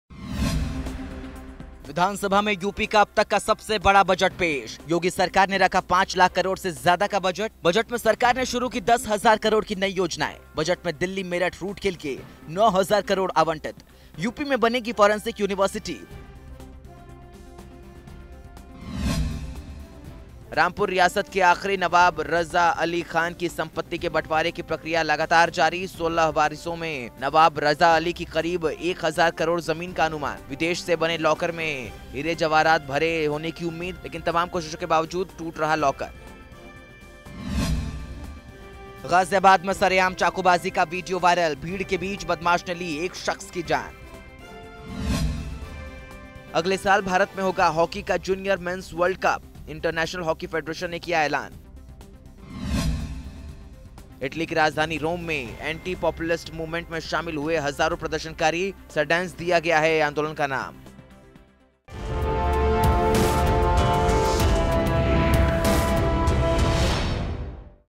2 बज गए: News Tak Top 5
विधानसभा में यूपी का अब तक का सबसे बड़ा बजट पेश...योगी सरकार ने रखा 5 लाख करोड़ से ज्यादा का बजट..बजट में सरकार ने शुरू की 10 हजार करोड़ की नई योजनाएं...बजट में दिल्ली मेरठ रूट के लिए 900 करोड़ आवंटित...यूपी में बनेगी फॉरेसिंक यूनिवर्सिटी...तलाशशुदा महिलाओं को मिलेगी आर्थिक मदद...